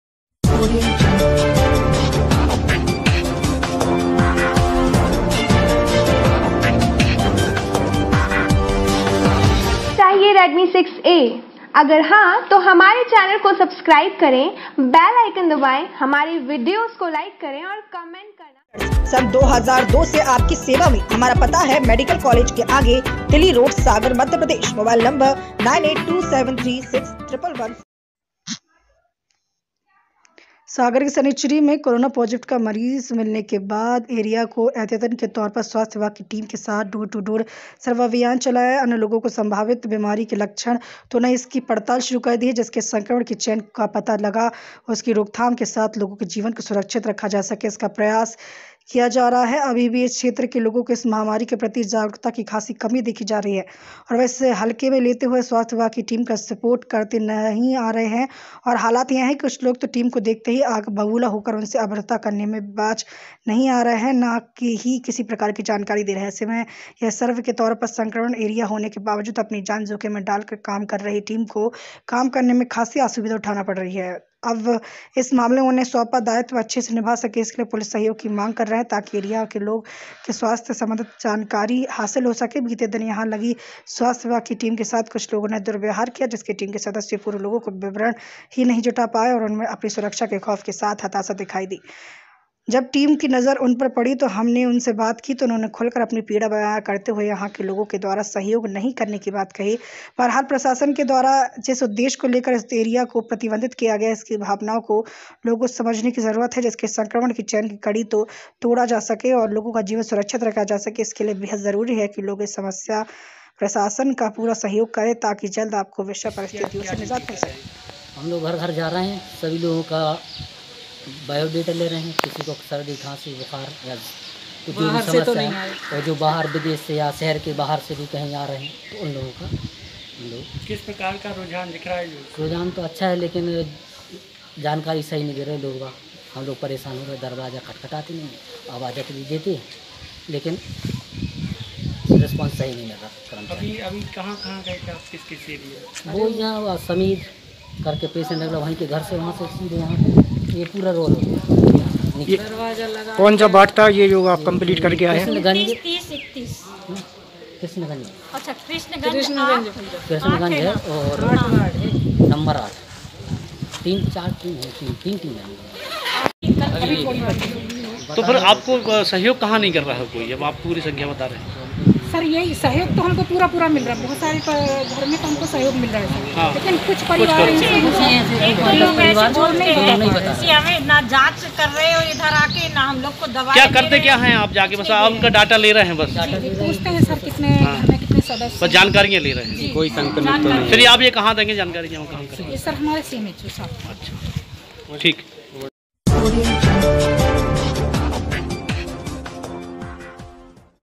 सागर के शनिचरी मे कोरोना पॉजिटिव का मरीज मिलने के बाद प्रशासन ने पूरे क्षेत्र को कंटेनमेंट एरिया धोष
खासी असुविधा उठाना पड रही ओर वह अब इस मामले मे उन्हे सौपा दायित्व अच्छे से निभा सके इसके लिए पुलिस सहयोग की मांग कर रहे ताकी इस एरिया के लोगो के स्वास्थ्य से संबंधित सही जानकारी हासिल हो सके बीते दिन यहाँ लगी स्वास्थ्य विभाग की टीम के साथ कुछ लोगो ने दुर्व्यवहार किया जिससे टीम के सदस्यों पूरे लोगो का विवरण ही नही जुटा पाए ओर उनमे अपनी सुरक्षा के खौफ के साथ हताशा दिखाई दी जब हमारी टीम की नजर उन पर पडी ओर हमने उनसे बात की तो उन्होंने खुलकर अपनी पीड़ा बया करते हुए यहाँ के लोगो के द्वारा सहयोग नही करने की बात कही बहरहाल प्रशासन के द्वारा जिस उदेश्य को लेकर इस एरिया को प्रतिबंधित किया गया है उसकी भावनाओ को लोगो को समझाने की जरुरत है जिससे संक्रमण की चैन की कड़ी को तोडा जा सके ओर लोगो का जीवन सुरक्षित रखा जा सके इसके लिए बेहद जरुरी है की लोग इस समय प्रशासन का पूरा सहयोग करे ताकी जल्द आपको इस बिषय परिस्थितियों से निजात मिल सके।